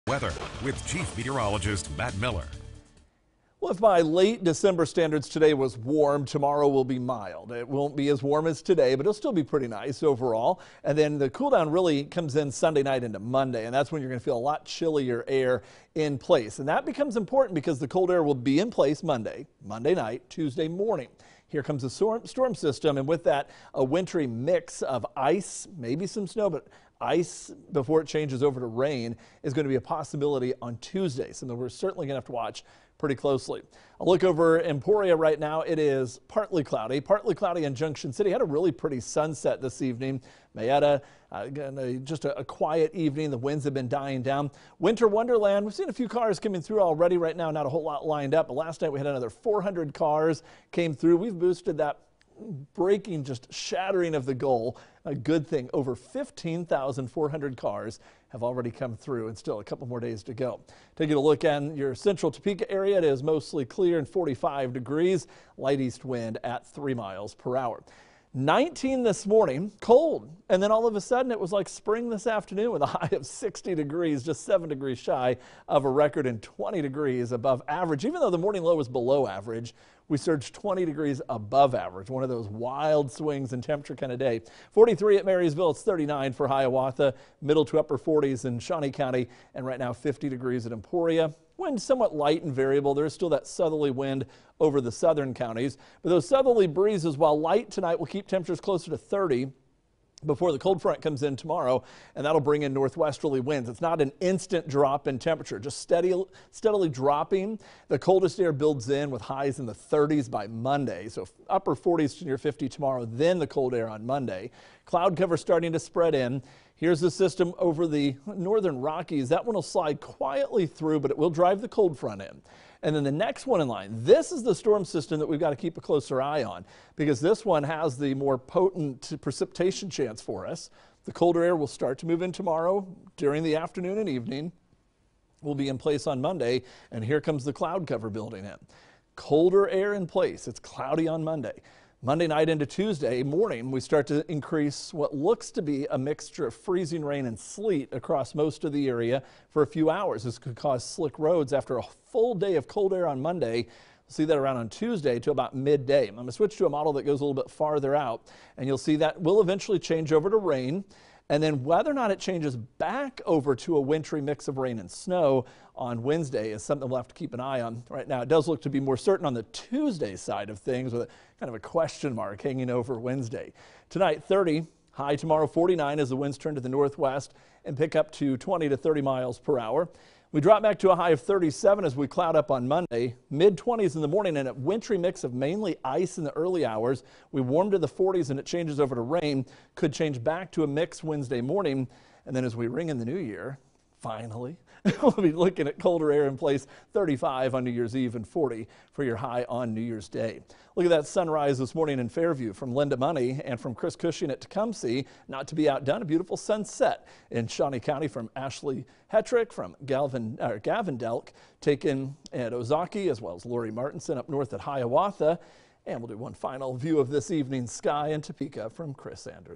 Latest Weather Forecast
Tracking a winter mix to start the system on Tuesday.